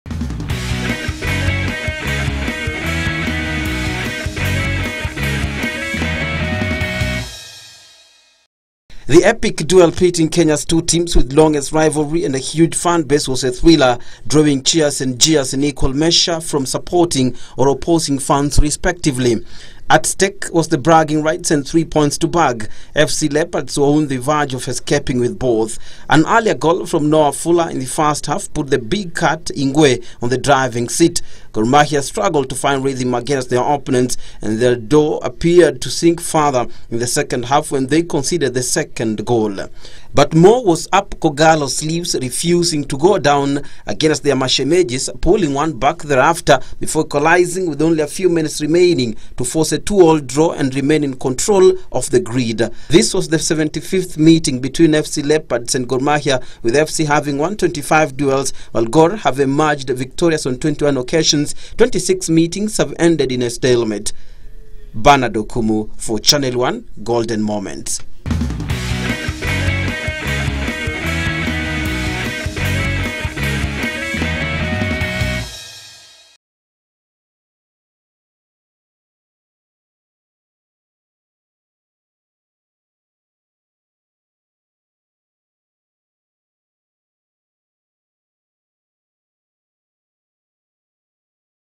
'Mashemeji derby' at Kasarani --GOLDEN MOMENTS
It  did live to its  billing as the biggest derby ever on the Kenya Premier League calendar, and indeed it was, what of Gor Mahia staging a stealth comeback in the dying minutes of the second half to snatch a draw against their opponents AFC Leopards. In our Golden Moments, we relieve the moments that were during the high octane 'Mashemeji Derby' at Kasarani